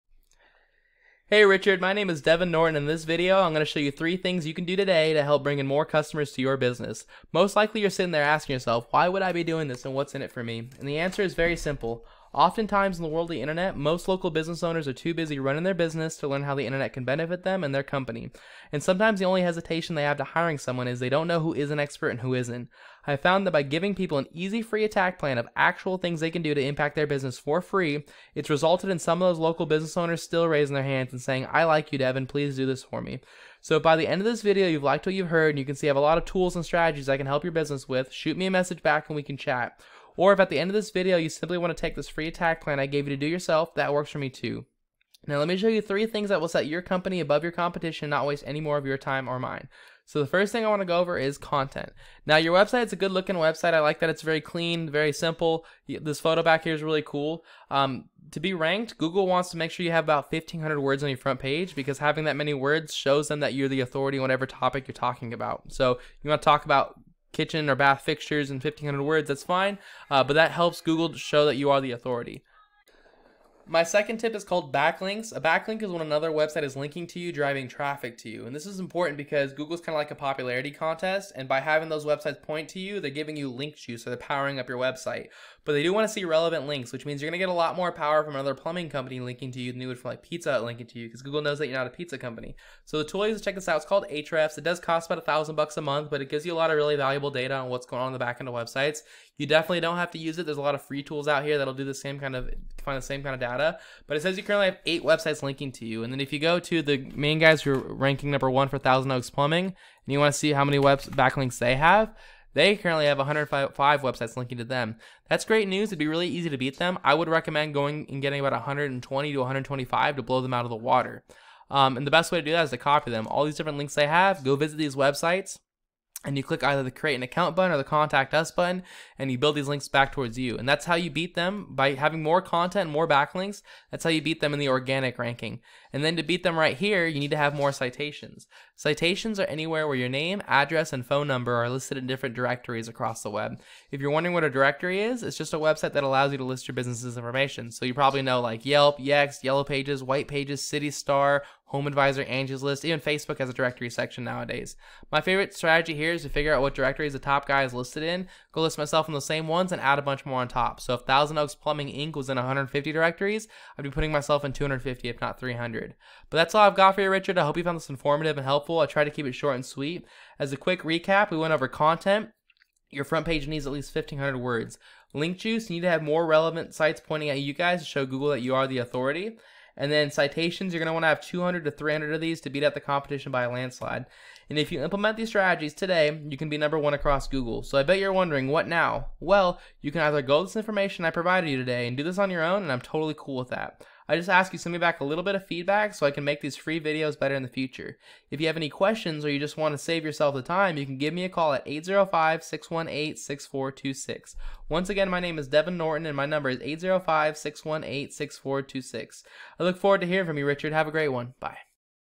Wildwood Specialty Plumbing, Thousand Oaks Plumbing
Hey Richard, Devin from PHL shows Wildwood Specialty Plumbing how easy it is to show up on the front page for Thousand Oaks Plumbing